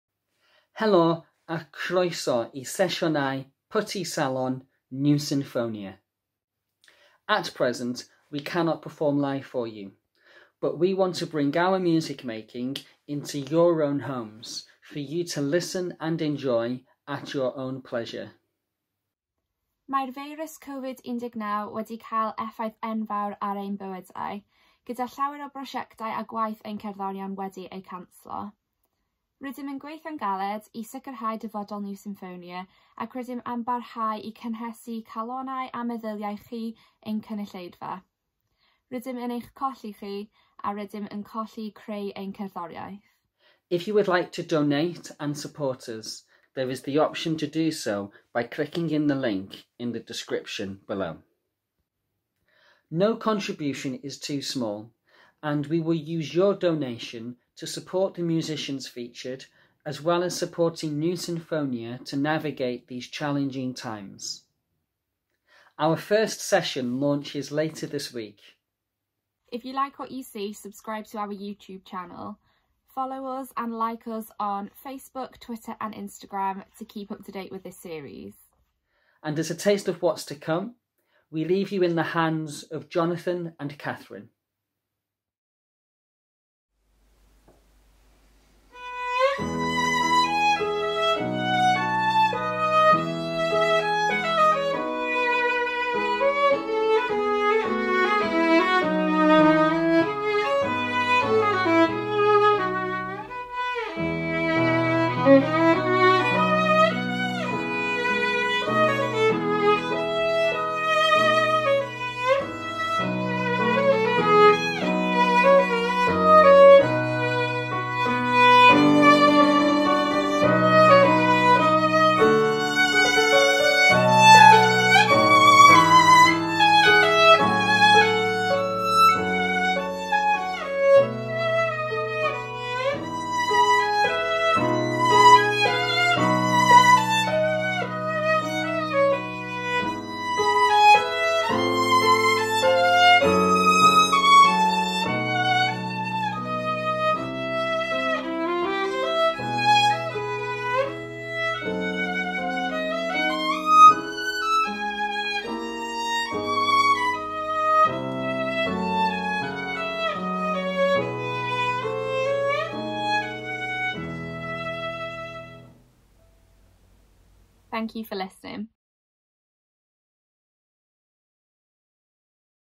Le Petit Salon Sessions Trailer
Le Petit Salon Sessions - bringing music to you from our homes

NEW Sinfonia present a new way for you to enjoy fantastic music performed by our musicians from the comfort of your own home.

Covid 19 is having a huge impact on our lives with many projects and concerts cancelled for our musicians. We are working hard to ensure the future of NEW Sinfonia and want to continue performing for you our audience.

No contribution is too small, and we will use your donation to support the musicians featured as well as supporting NEW Sinfonia to navigate these challenging times.

Thank you very much for your support and enjoy the music!